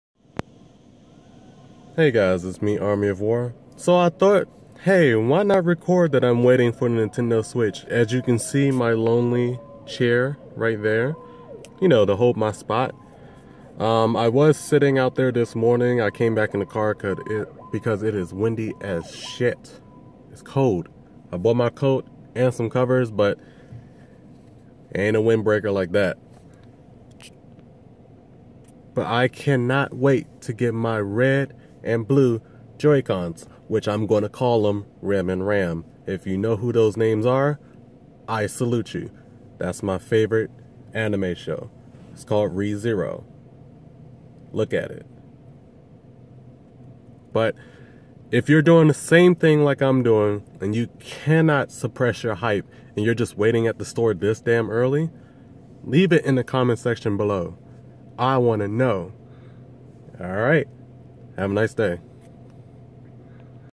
Waiting at Best Buy for the Nintendo switch!!!!! Hype #1
I cannot suppress my hype at all... its on 1,000% right now!!! I really want to play Zelda.

They're about to put up the Nintendo switch Gates!!!